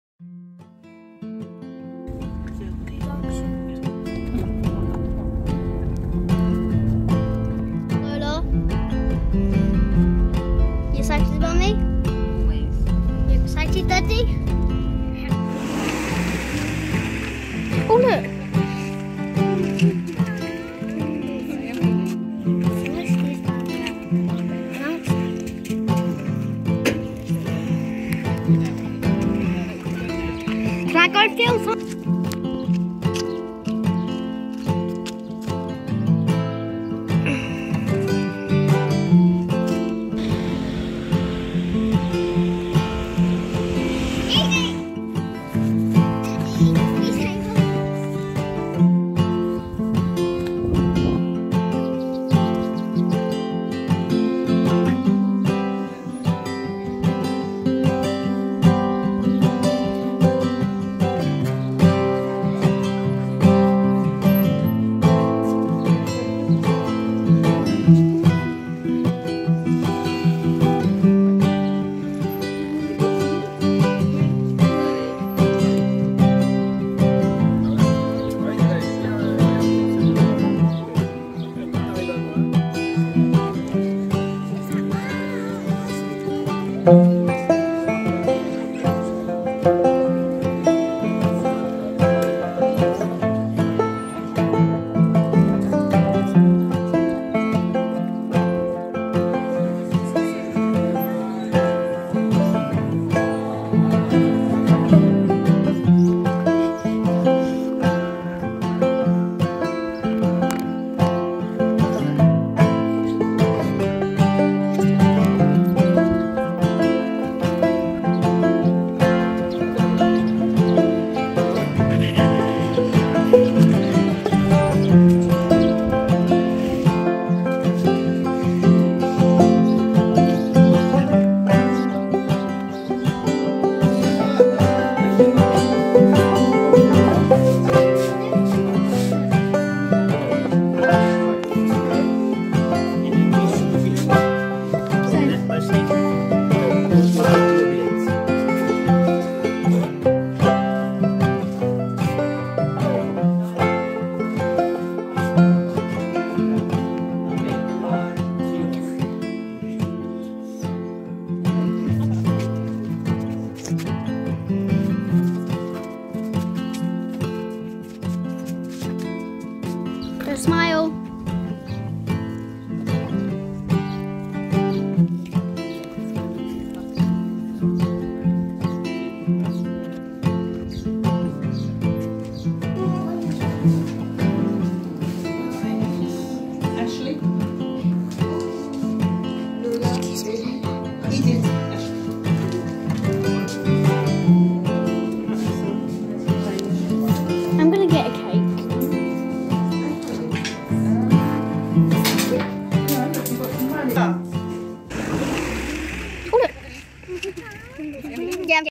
Edith and Lola went to a Brocante
Edith filmed her way round Chezal Benoits Brocante.